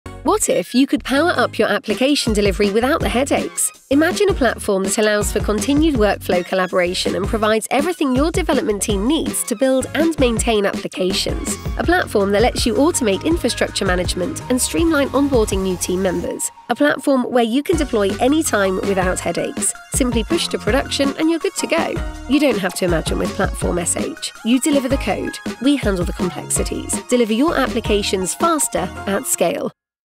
You deliver the code. We handle the complexities ✅
Save 25% dev time and boost app delivery*
According to Gartner, platform engineering “improves the developer experience and productivity by providing self-service capabilities with automated infrastructure operation.” At Platform.sh, we’ve done the platform engineering so you don’t have to.

Building and delivering one, ready-to-use platform which empowers your team to deploy efficiently, eliminate workflow bottlenecks, and reduce onboarding.

Start innovating.